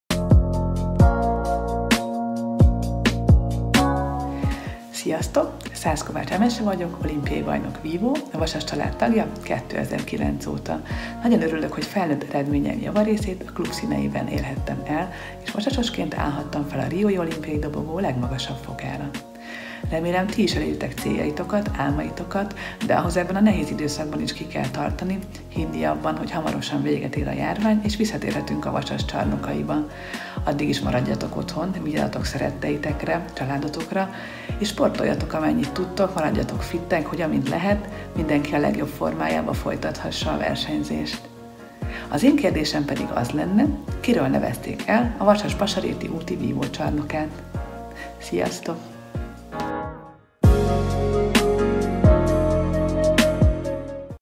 Vasas Társas-Kvízjáték 24.: Szász-Kovács Emese
Nem könnyű a mostani időszak, leállt a sportélet, nincsenek közös edzések, mi viszont szeretnénk az otthonmaradást minél jobban vasasossá tenni. Ennek jegyében társas-kvízjátékot indítunk a Vasas család tagjai számára, amelyben április végéig minden nap kérdéseket tesznek fel Nektek klubunk sportolói. A helyes válaszadók értékes piros-kék nyereményekkel gazdagodhatnak. Jó játékot mindenkinek!